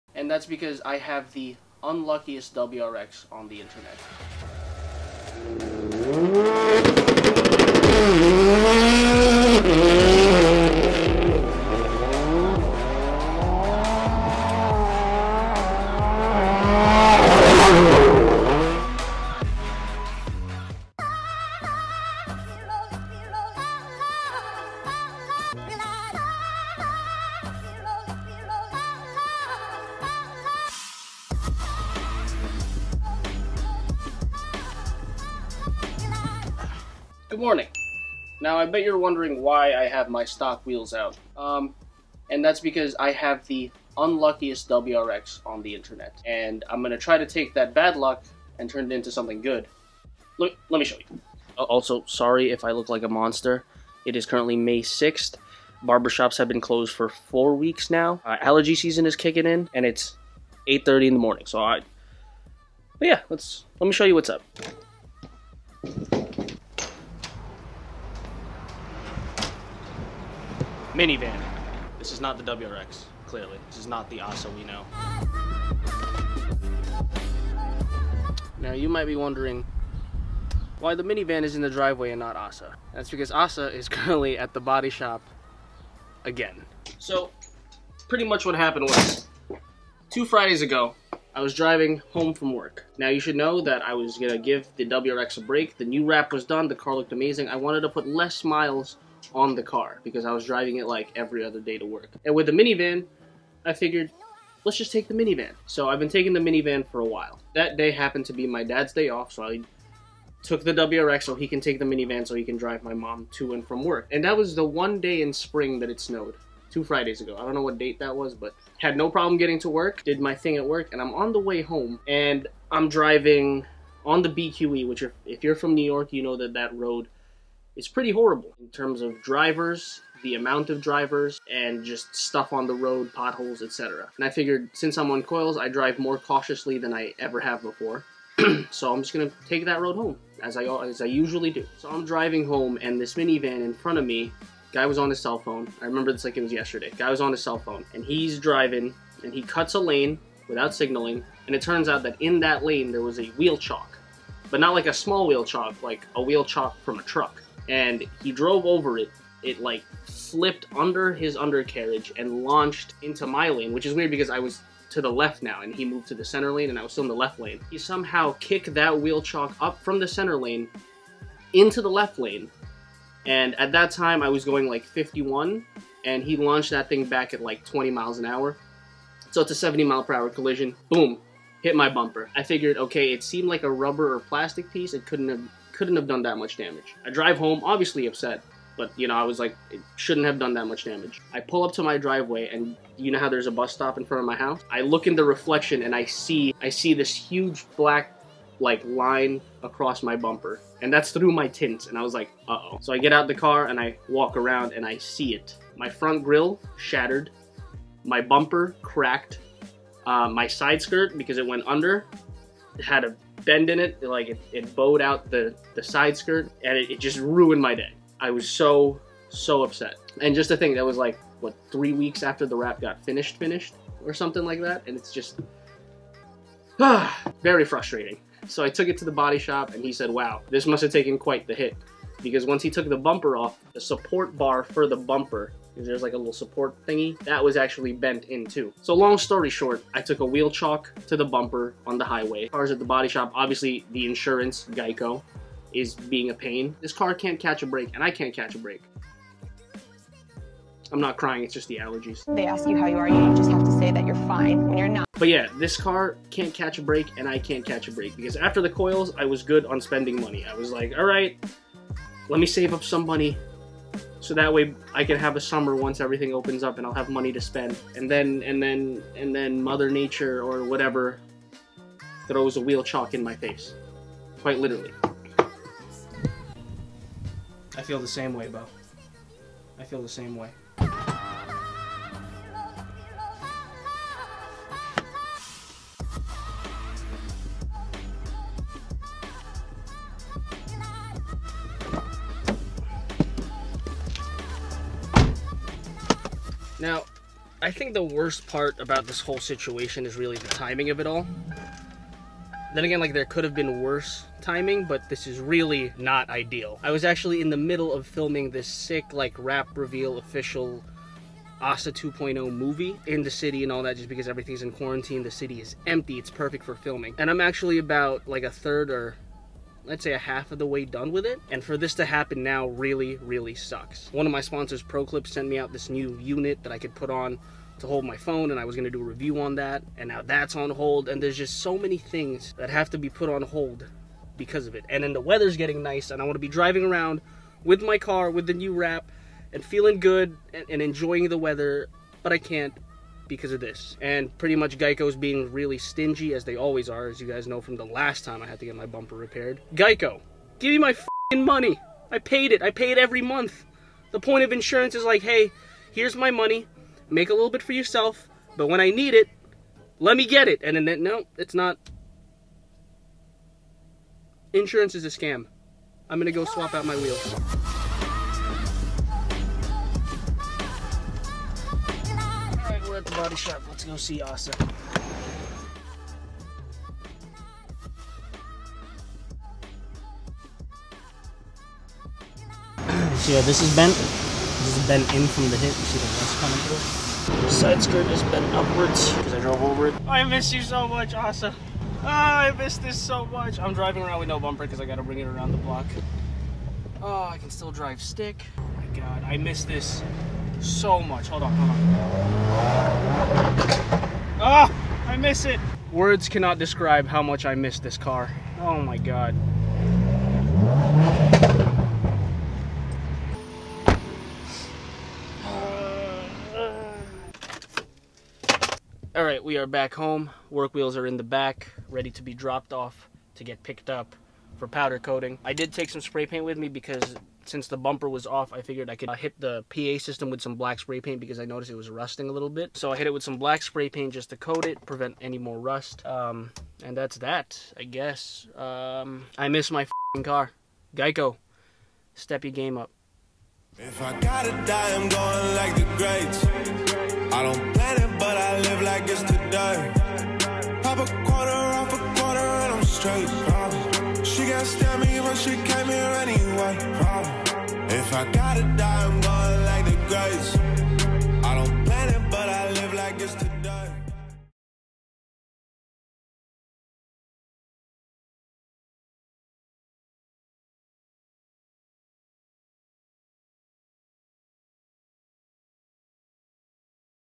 the UNLUCKIEST WRX on YouTube...
I'm pretty sure my WRX is cursed... because she is down AGAIN... This is irrefutable proof that I have the Unluckiest WRX on youtube...

Got Some Business for Me?